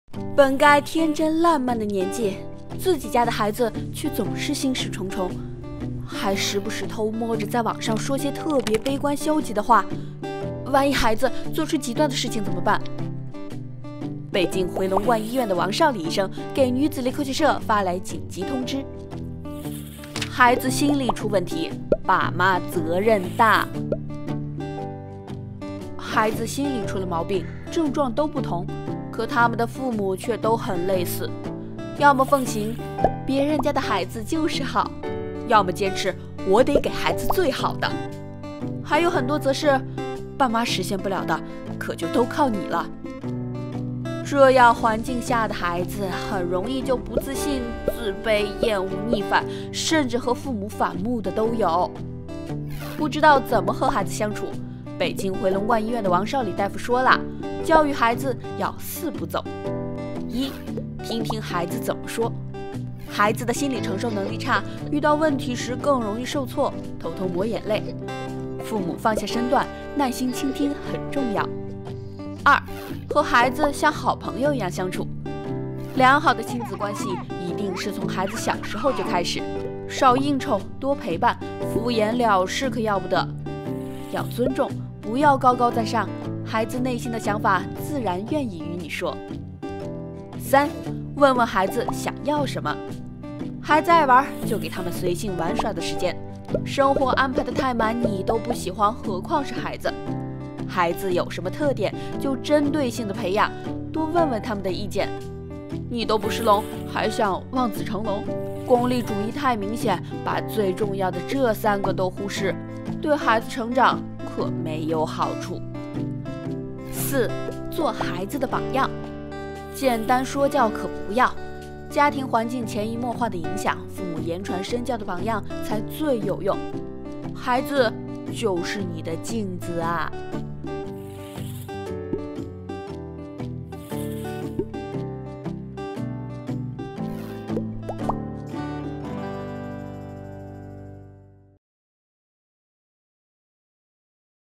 【女子力科学社】孩子心理出问题，都是爸妈不会好好说话
有部分孩子，他们性格孤僻，不爱与人说话，也有的叛逆起来连你妈妈都要骂。孩子们的心理症状大不一样，但他们的父母都很一样。很多人看了都唏嘘黄圣依和她儿子的相处太...尴尬！ “忙，没时间照顾孩子”，孩子越来越抵触和妈妈相处，甚至连陌生人都不如。北京回龙观医院的王绍礼大夫致电@女子力科学社，孩子心理有问题，爸妈的责任最大！